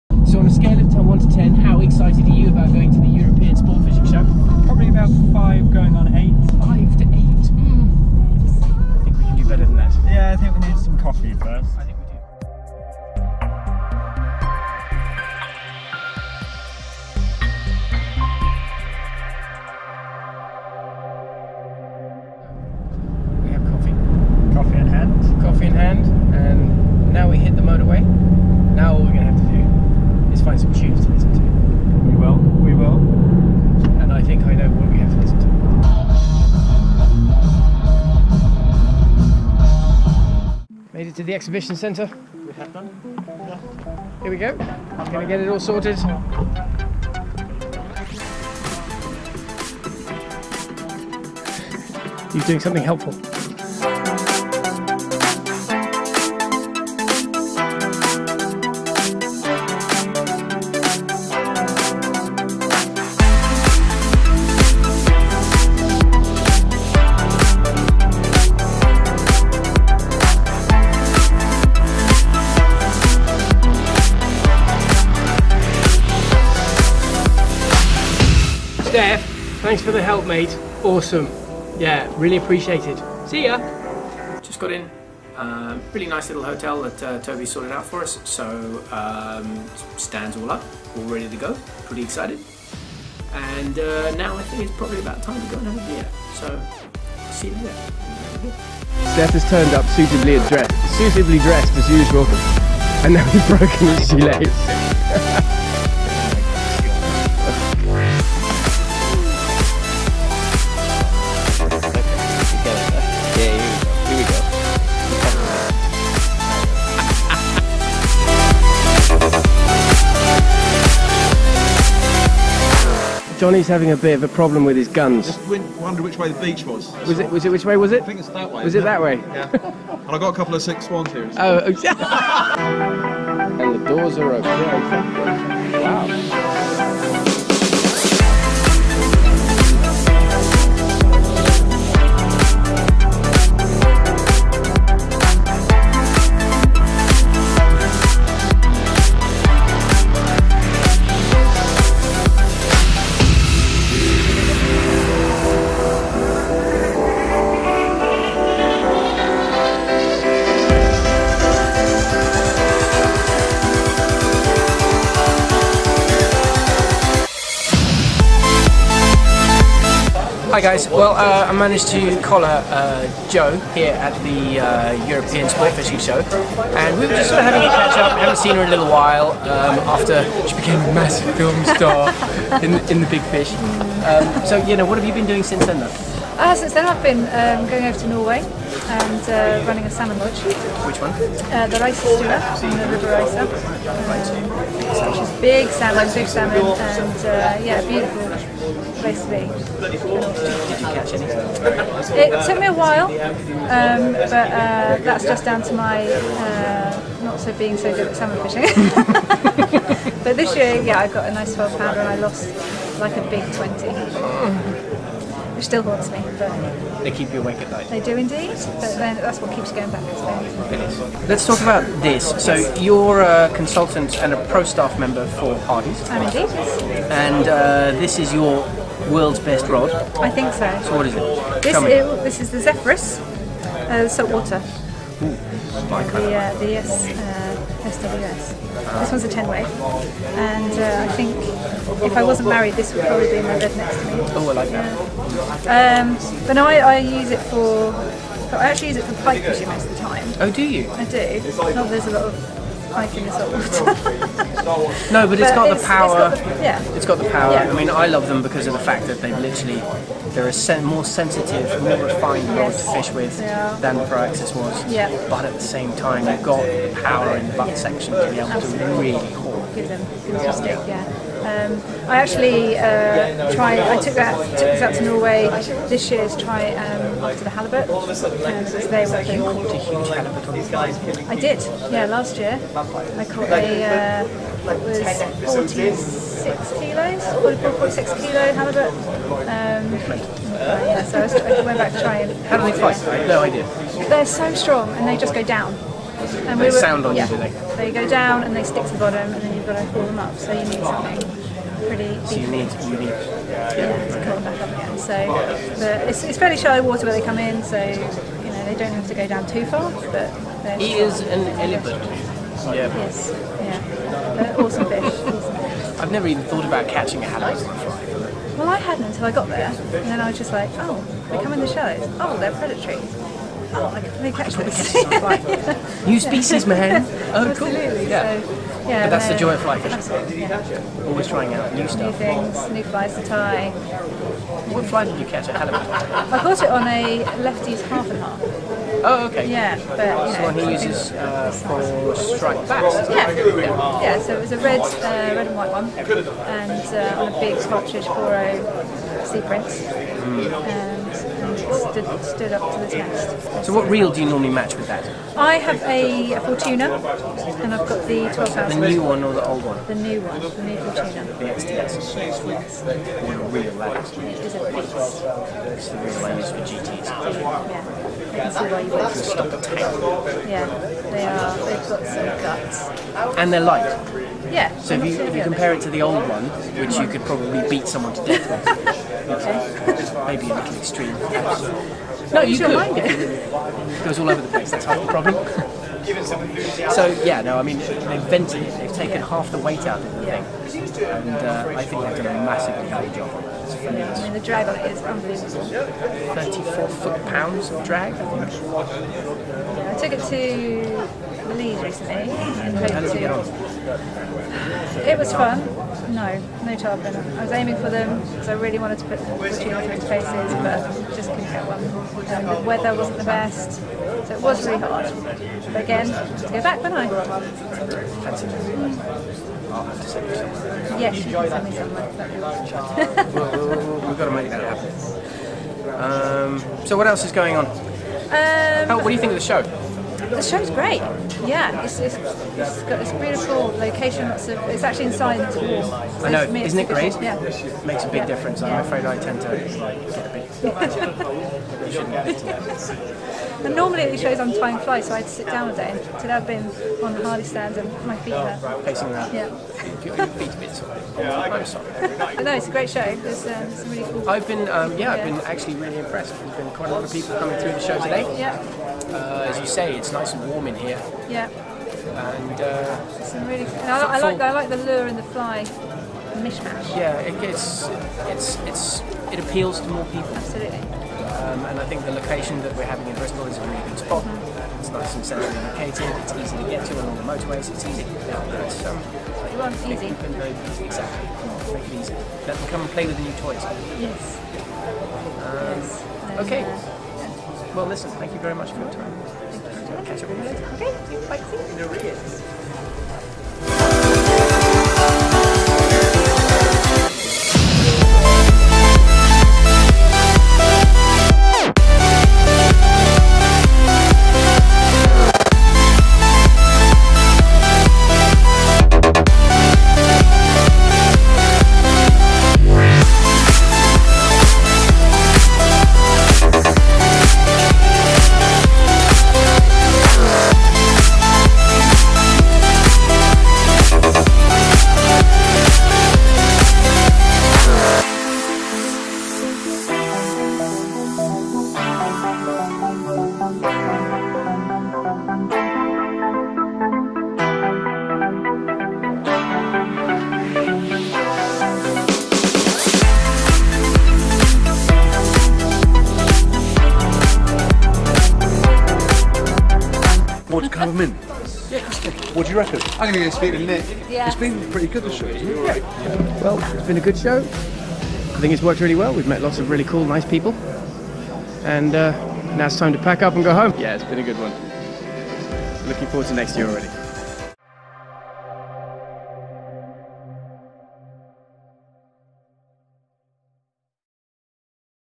European Sportfishing Show 2017 Review
In November 2017 Aardvark McLeod sponsored and attended the European Sportfishing Show in Bristol. This was its first year and it is an excellent venue, and this short vlog will give an insight to the show. Many top names in the industry attended and we look forward to returning next year as it grows. The show covers not only fly fishing, but also lure fishing and next year sea angling. Huge thanks to Toby Merigan for organising it.